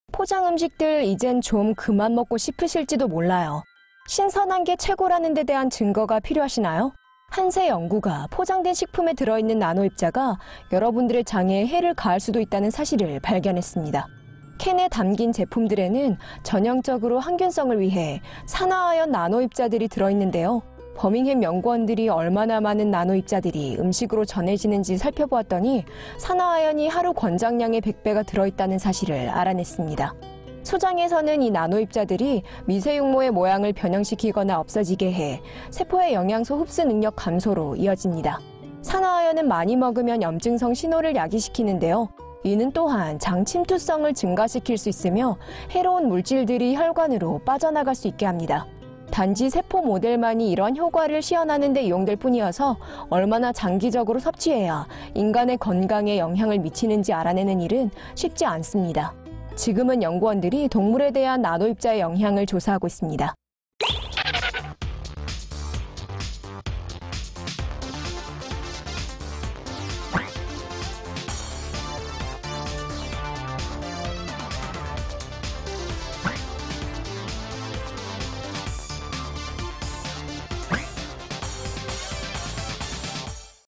포장제품들, 장에 위협을 가하고 있을지도
뉴욕, 버밍햄 — 패키지 음식들, 이젠 좀 그만 먹고 싶으실지도 몰라요.

신선한게 최고라는데 대한 증거가 필요하시나요? 한 새 연구가 포장된 식품에 들어있는 나노입자가 여러분들의 장에 해를 가할 수도 있다는 사실을 발견했습니다.

캔에 담겨진 제품들에는 전형적으로 항균성을 위해 산화아연 나노입자들이 들어있습니다.

버밍햄 연구원들이 얼마나 많은 나노입자들이 음식으로 전해지는지 살펴보았더니, 산화아연이 하루권장량의 100배가 들어있다는 사실을 알아냈습니다.

소장에서는 이 나노입자들이 미세융모의 모양을 변형시키거나 없어지게 해, 세포의 영양소 흡수능력 감소로 이어집니다.

산화아연은 많이 먹으면 염증성 신호를 야기시킵니다. 이는 또한 장 침투성을 증가시킬 수 있으며, 해로운 물질들이 혈관으로 빠져나갈 수 있게 합니다.

단지 세포 모델만이 이러한 효과를 시연하는데 이용될 뿐이어서, 얼마나 장기적으로 섭취해야 인간의 건강에 영향을 미치는지를 알아내는 일은 쉽지 않습니다.

지금은 연구원들이 동물에 대한 나노입자의 영향을 조사하고 있습니다.